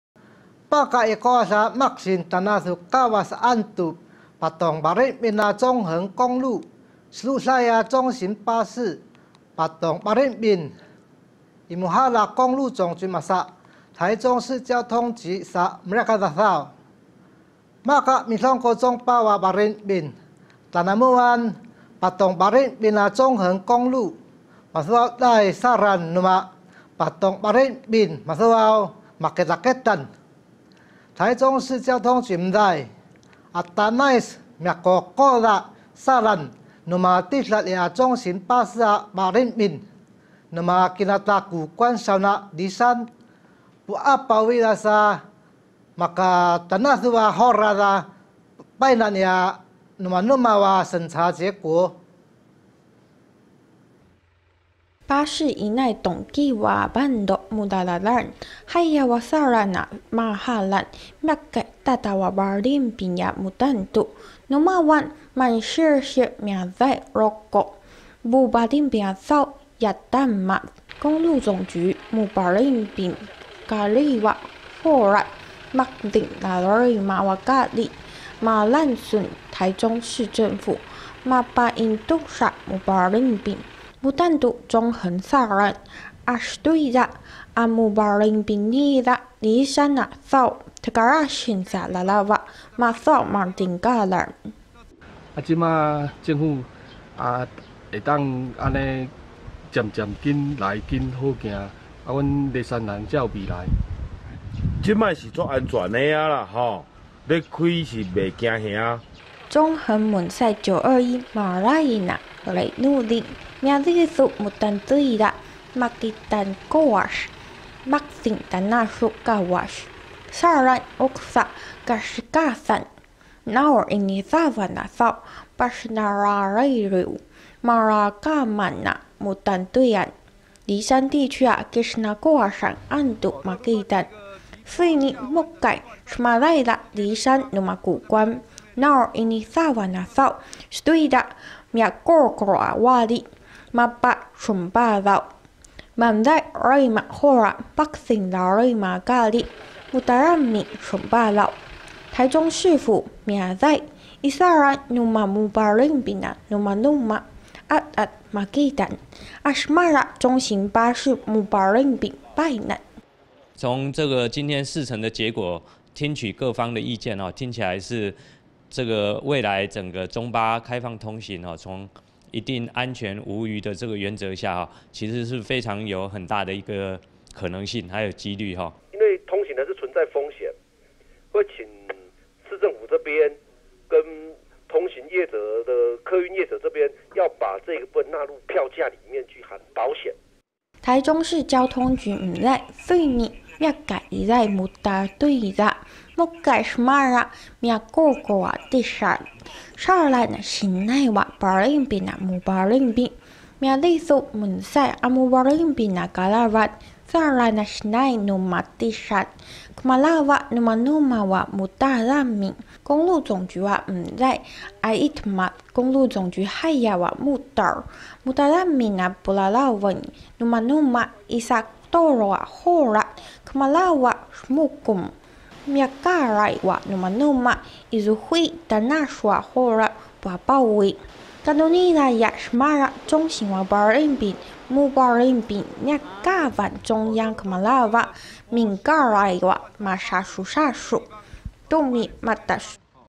中斷近19年 中橫公路會勘有望開放中巴 2018-06-09 Thau IPCF-TITV 原文會原視族語新聞
mutuniza makitnaqualh 19 kawash 中橫 saran alhmaza 中巴 painan

巴士在轉彎處緩緩前進，這段路寬只夠一輛車通過，而且旁邊就是山谷，司機特別小心，公路總局5月15日會同台中市政府，分別搭乘2輛中型巴士進入中橫便道，要評估是否開放一般車輛通行，梨山當地的居民聽到消息，都相當開心。

中橫從921大地震後已經中斷將近19年，道路一修再修，當地居民抱怨交通不便，影響梨山地區的觀光，目前僅開放梨山和谷關居民能在固定時間通行，不過5月15日的試行，台中市府認為路況和行車狀況都不錯，有望開放中型巴士通行。

台中市交通局表示，目前大方向仍朝僅開放固定時間、路線的中型巴士通行，已經針對搭乘方式、路線、時間，著手進行相關評估規劃，公路總局的建議，也將會納入。公路總局將把這次的試行所蒐集的資料，於6月進行審查，最後結果會在9月出爐，但是否開放中巴通行，仍須中央做最後決議。